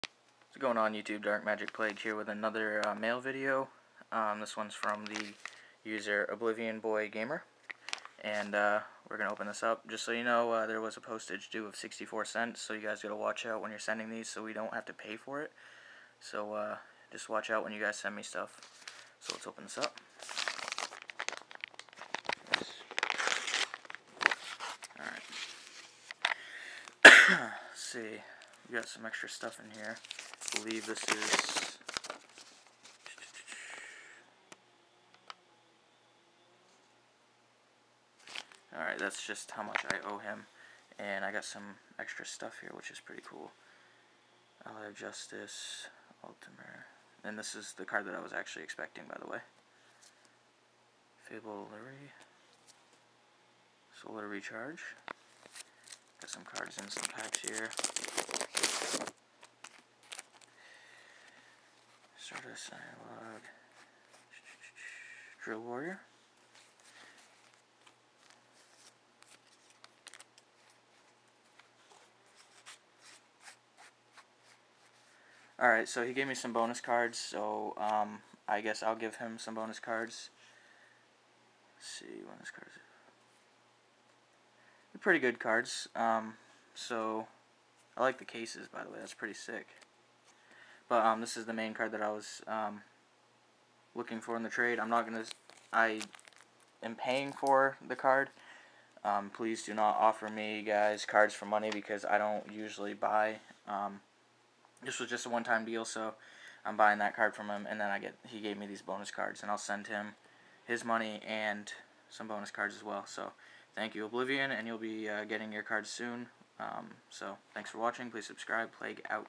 Yu-Gi-Oh Mail Video #4
Trade from the user : OblivionBoyGamer

Sent : Korean Dark Magician Alternate Artwork (Super)

Trade : Money

This was a one time deal. I only trade. If you want to trade please message me and we can figure something out.